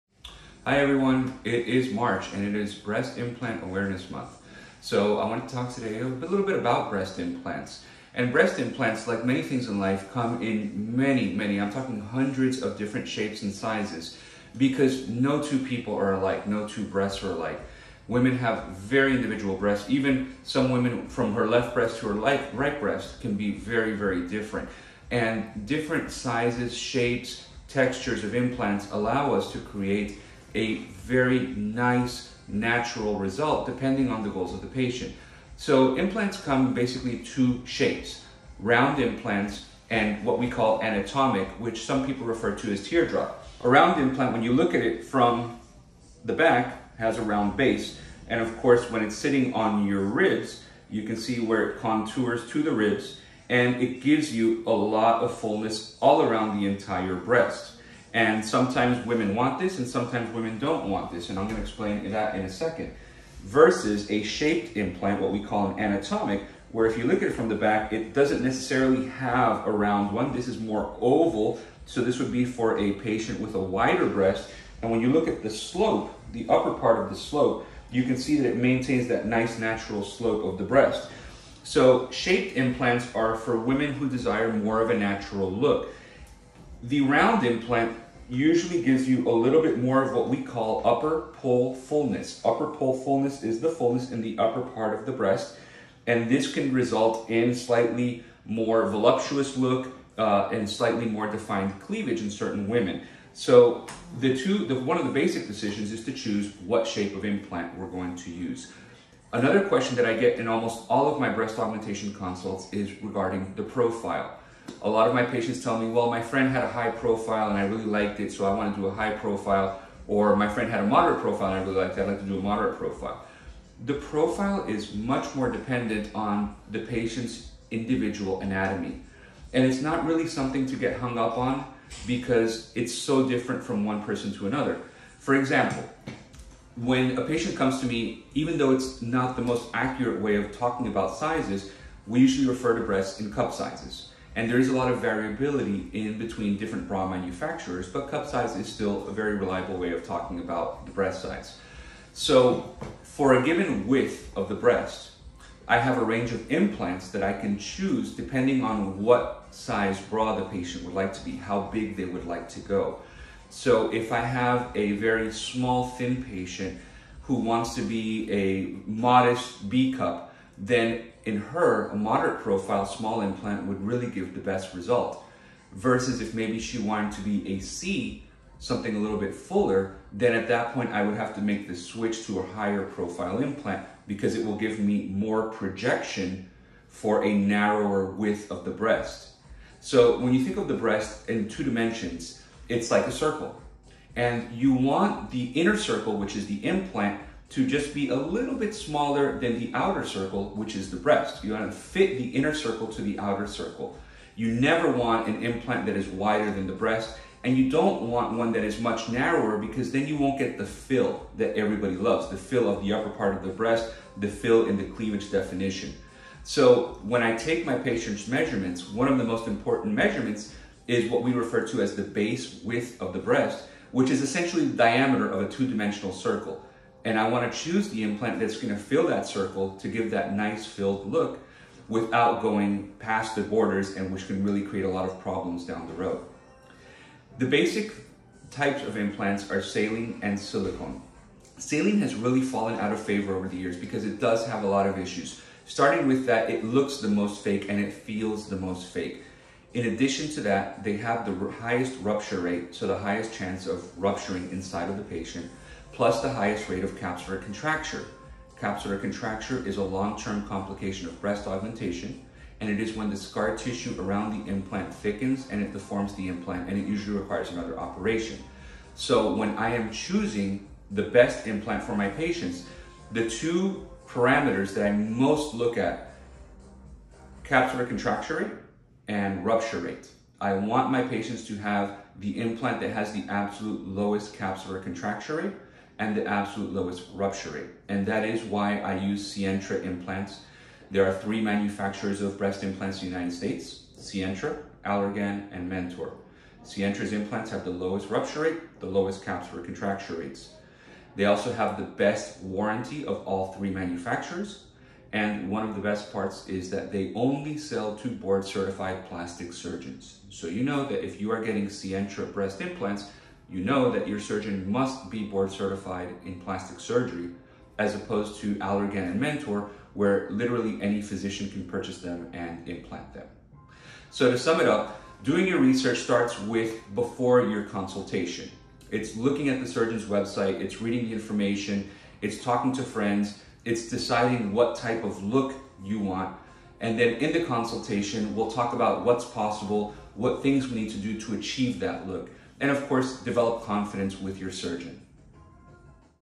Let's talk about Breast Implants!
All you need to know about breast implants!
Our Double Board Certified Plastic Surgeon, Dr. Daniel Careaga, wanted to explain the different types of breastimplants and why he trusts and prefers the Sientra implants.
If you learn more about breastaugmentation and our practice, visit our